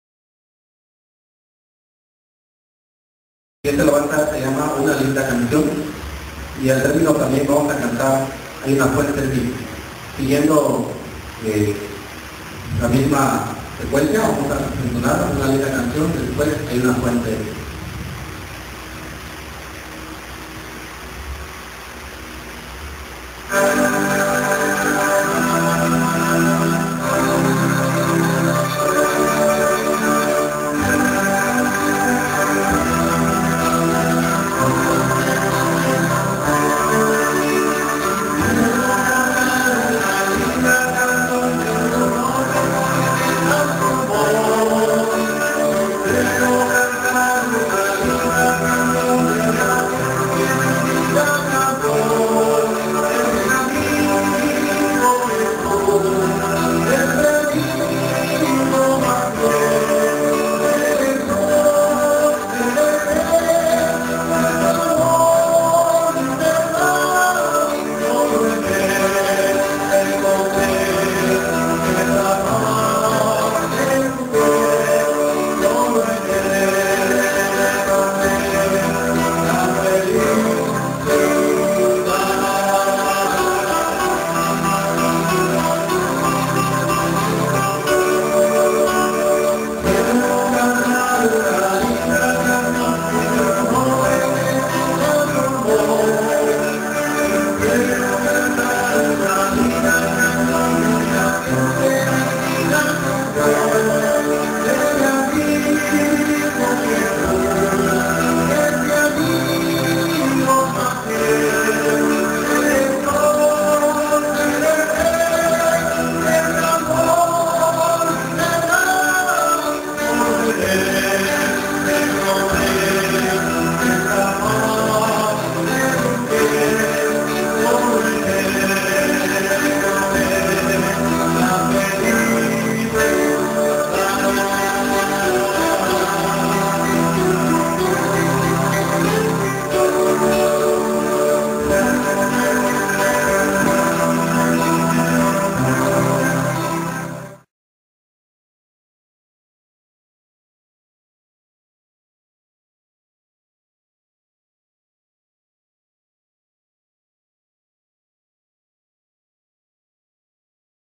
Una linda cancion por Ministerio El-Elyon
Previo ensayo, Iglesia presbiteriana Fuente de vida eterna, Tapachula, Chiapas. Col. Colinas del Rey.
Agradecimientos a Miguel Diaz en los sonidos y grabación.